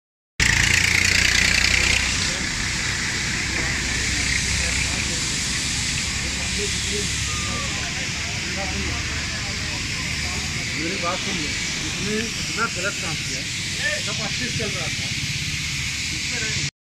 Ferther Woods representive (Azim) being arrested in case of fraud, robbery and threating. (Bangalore
Ferther Woods representive (Azim) being arrested in case of fraud, robbery and threating.